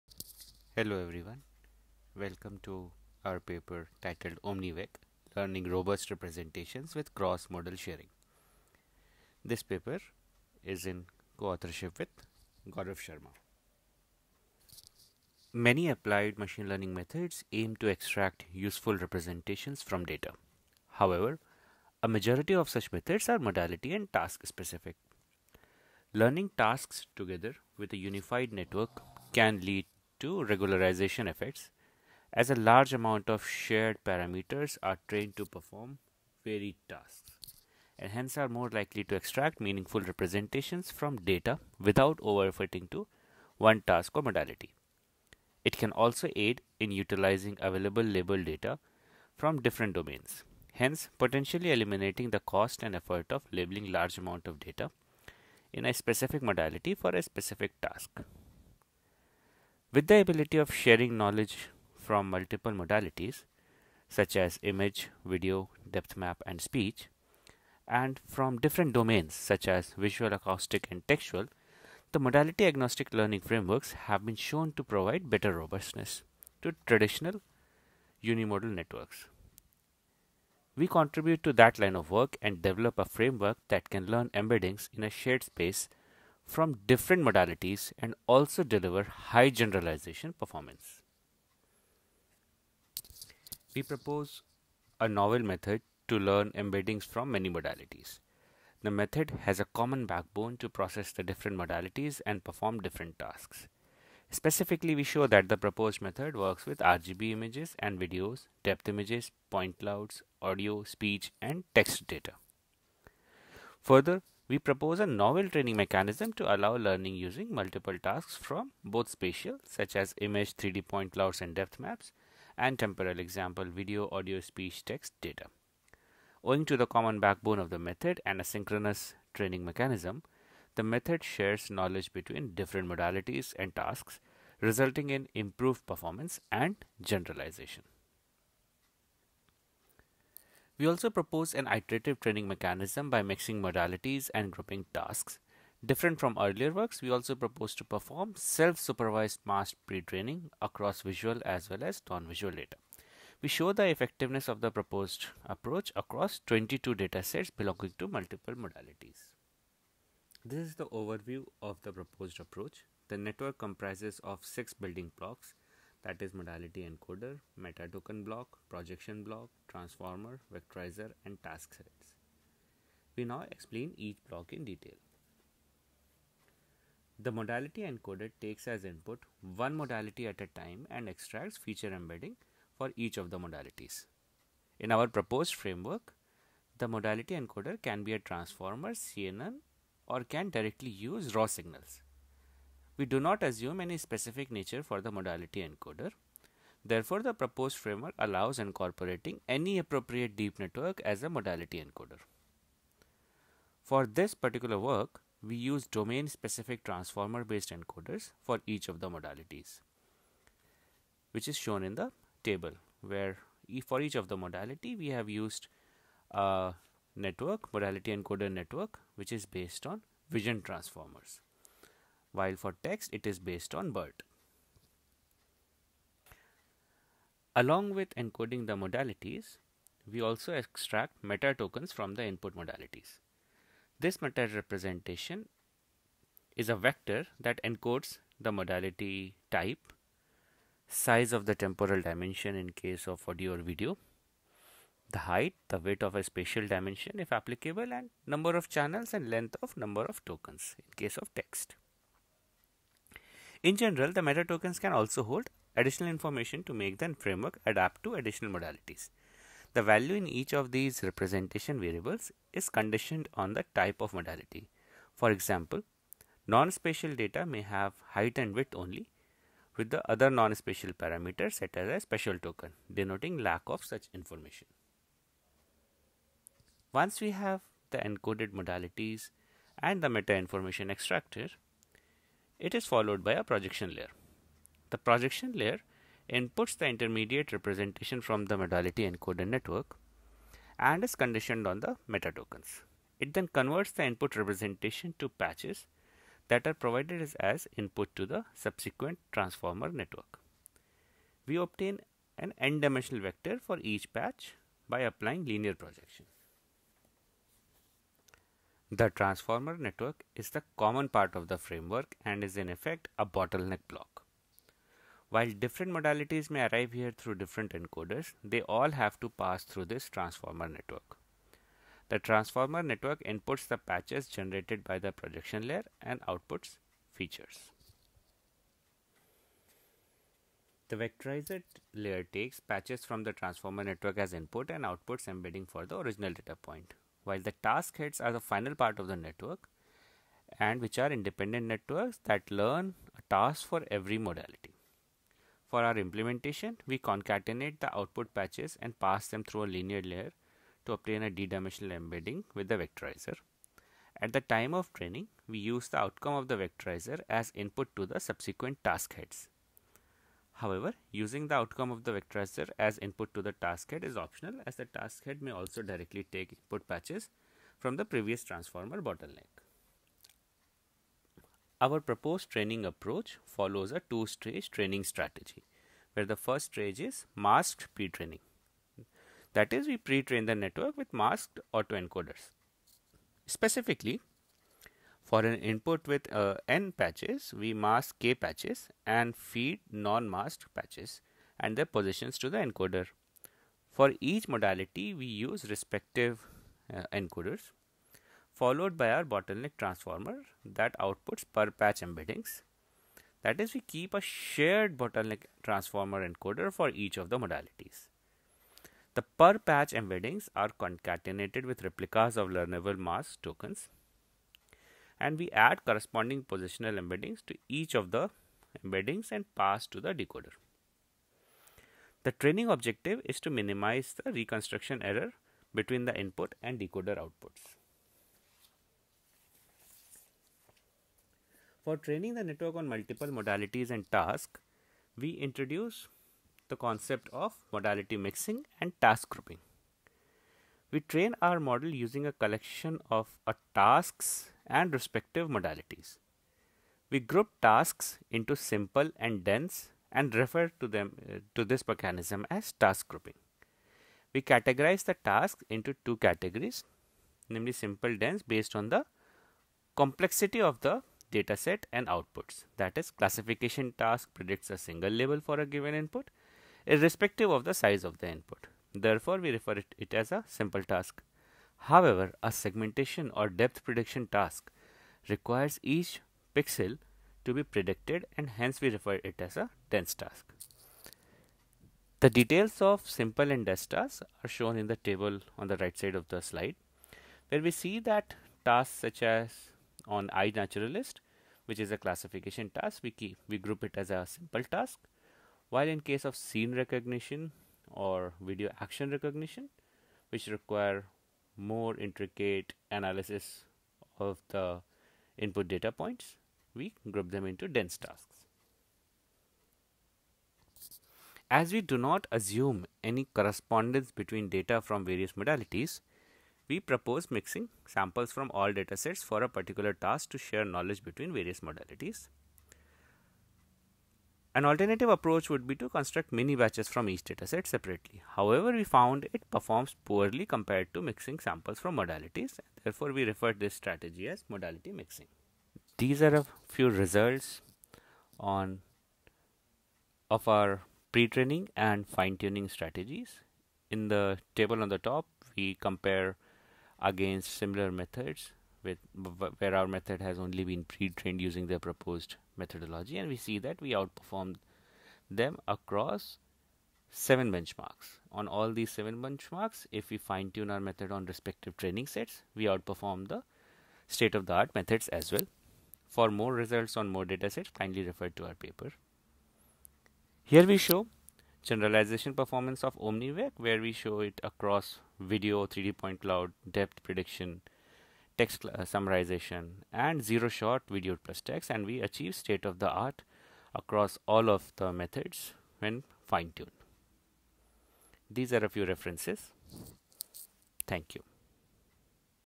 OmniVec: Learning Robust Representations With Cross Modal Sharing
Authors: Siddharth Srivastava; Gaurav Sharma
 Description: Majority of research in learning based methods has been towards designing and training networks for specific tasks. However, many of the learning based tasks, across modalities, share commonalities and could be potentially tackled in a joint framework. We present an approach in such direction, to learn multiple tasks, in multiple modalities, with a unified architecture. The proposed network is composed of task specific encoders, a common trunk in the middle, followed by task specific prediction heads. We first pre-train it by self-supervised masked training, followed by sequential training for the different tasks.

We train the network on all major modalities, e.g. visual, audio, text and 3D, and report results on 22 diverse and challenging public benchmarks. We demonstrate empirically that, using a joint network to train across modalities leads to meaningful information sharing and this allows us to achieve state-of-the-art results on most of the benchmarks. We also show generalization of the trained network on cross-modal tasks as well as unseen datasets and tasks.